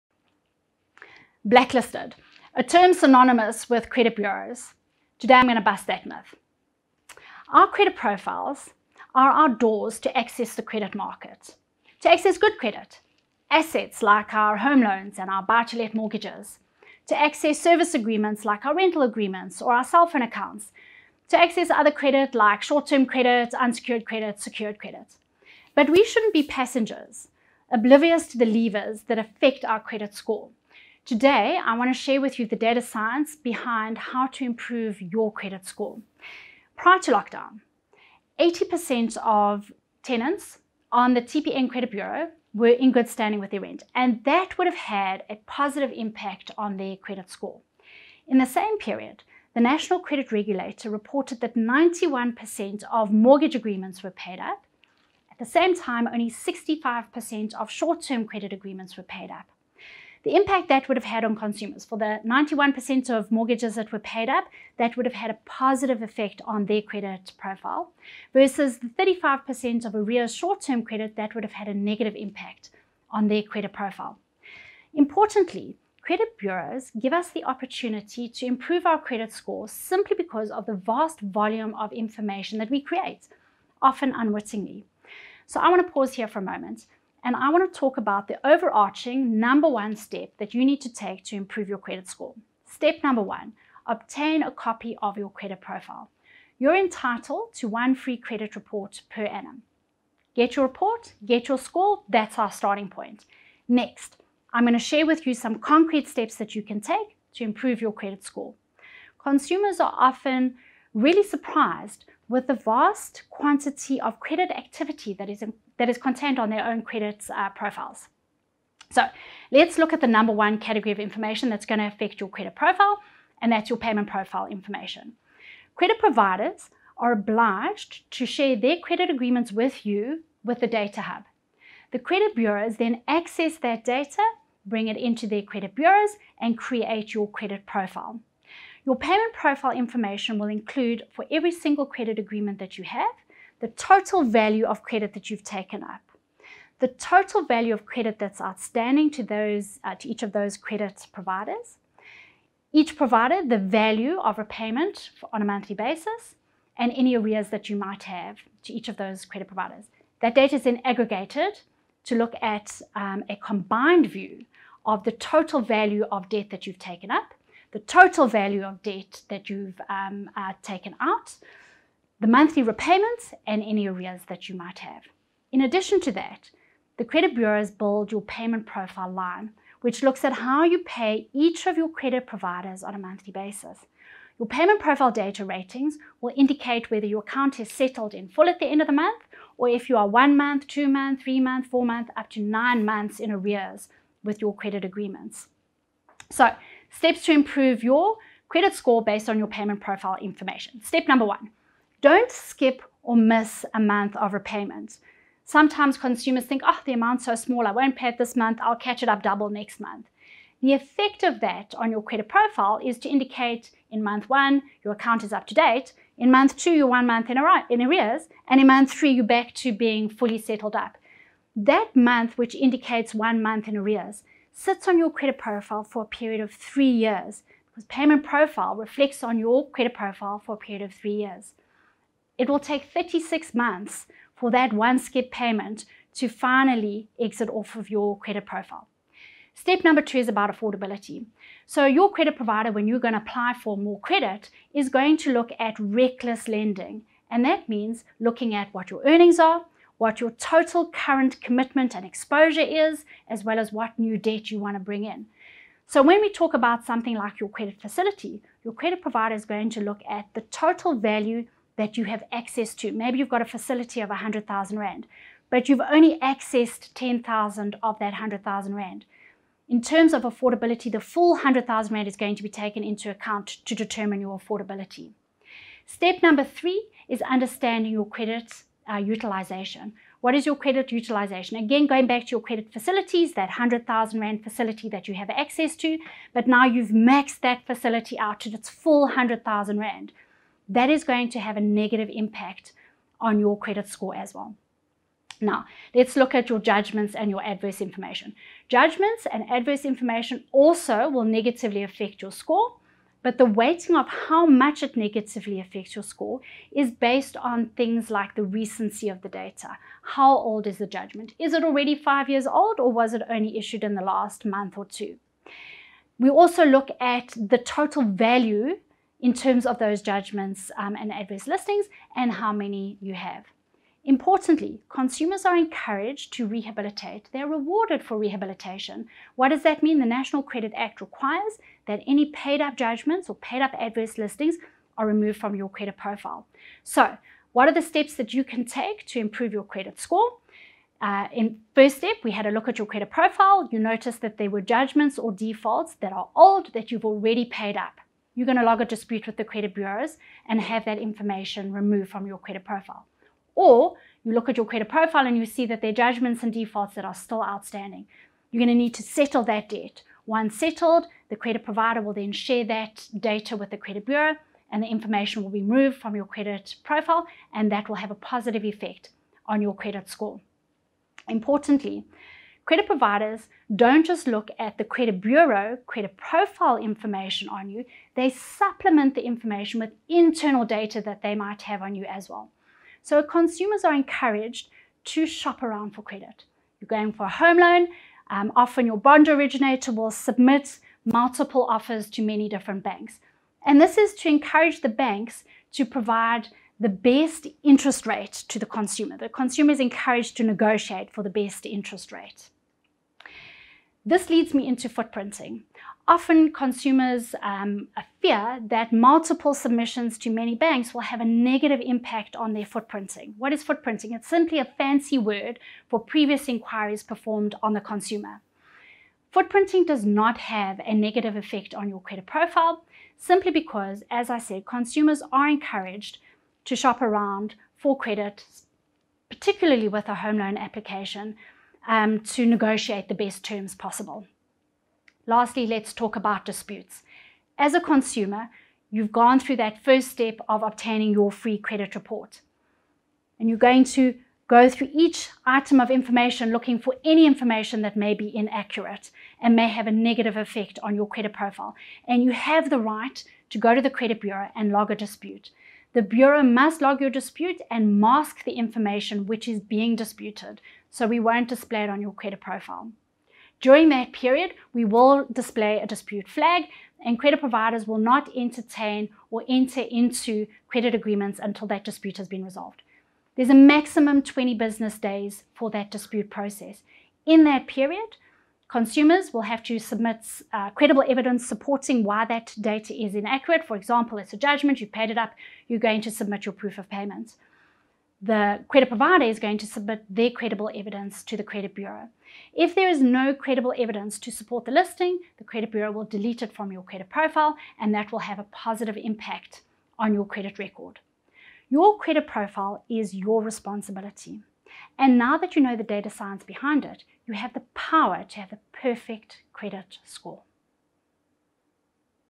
The data science behind your perfect credit score | VPS Reloaded
The data science behind your perfect credit score

Get to know your credit score:
• You are entitled to 1 free credit report every year
• Never skip or miss a payment – the impact lasts 36 months
• You are rewarded for rehabilitation – pay up old debt and it will be deleted from your credit profile

Presented by, Michelle Dickens, Managing Director, @TPNCreditBureau